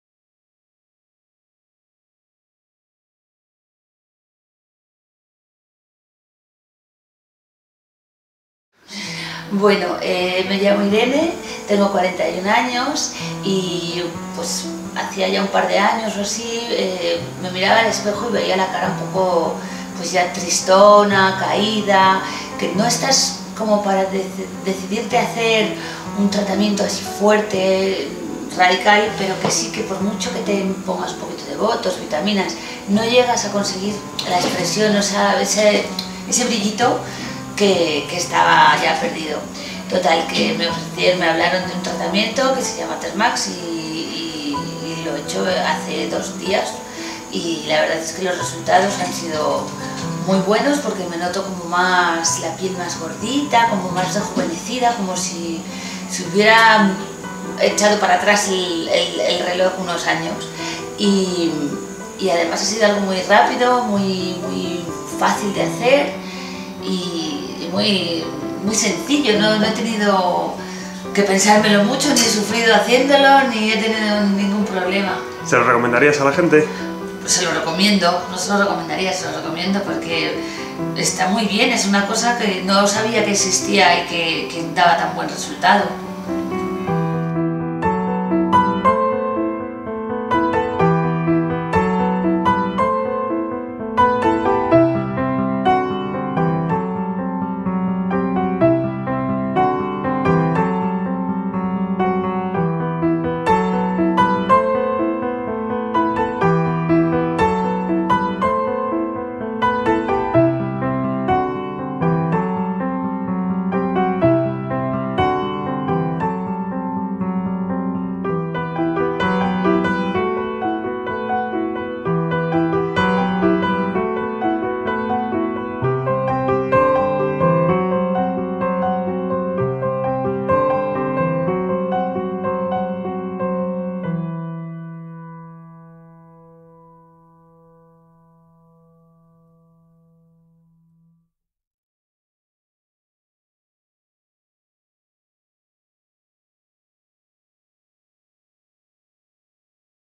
Thermage Antes y Después | Testimonios Clínicas Diego de León
Irene su testimonio de Thermage Antes y Después del tratamiento en nuestras Clínicas Diego de León.

La radiofrecuencia Thermage es la solución para eliminar la flacidez y la celulitis en cara, cuello, brazos, abdomen y piernas.

No te pierdas este vídeo si quieres rejuvenecer el aspecto de tu piel y ver el resultado del Thermage Antes y Después de Irene.

Teléfono Gratuito: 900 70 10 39

TESTIMONIO:

Me llamo Irene, tengo 41 años y pues hacía ya un par de años o así me miraba al espejo y  veía la cara pues ya tristona, caída que no estás como para decidirte a hacer un tratamiento así fuerte, radical, pero que por mucho que te pongas un poquito de bótox y vitaminas, no llegas a conseguir la expresión, ese brillito que estaba ya perdido.

Total que me hablaron de un tratamiento que se llamaba thermage y lo he hecho hace dos días y la verdad es que los resultados han sido muy buenos porque me noto como la piel más gordita como más rejuvenecida, como si se hubiera echado para atrás el reloj unos años. Y además ha sido algo muy rápido, muy fácil de hacer y muy sencillo, no he tenido que pensármelo mucho, ni he sufrido haciéndomelo ni he tenido ningún problema.

- ¿Se lo recomendarías a la gente?

Se lo recomiendo porque está muy bien, es una cosa que no sabía que existía y que daba tan buen resultado.